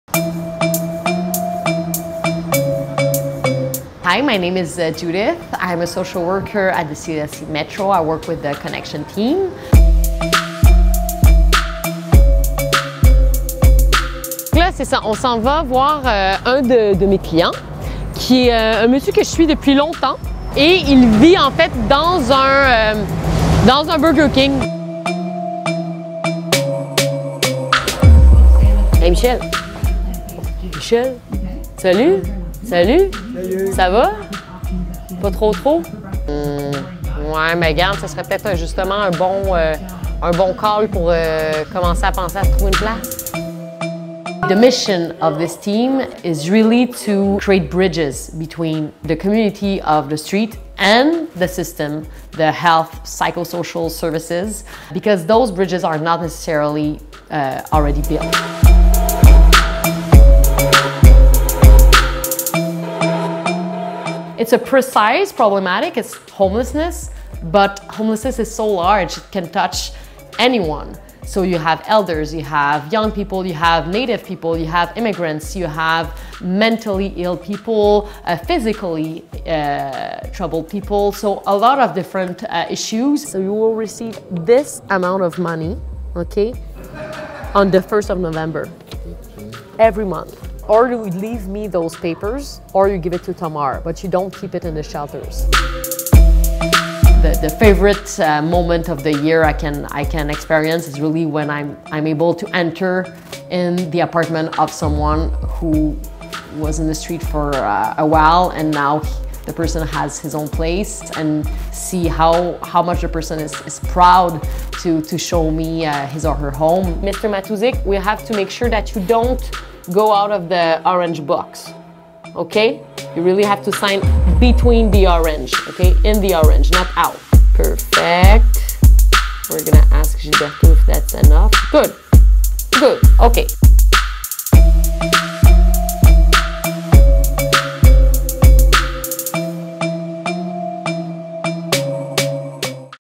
CLSC Métro Connexion Team: Meet Judith Sigouin, Street Social Worker
The Connexion team of CIUSSS West-Central Montreal provides help to the homeless.

Judith Sigouin, social worker at CLSC Métro, is a member of the team. She is the link between the social services system and people living without a fixed address.

We followed Judith on the ground.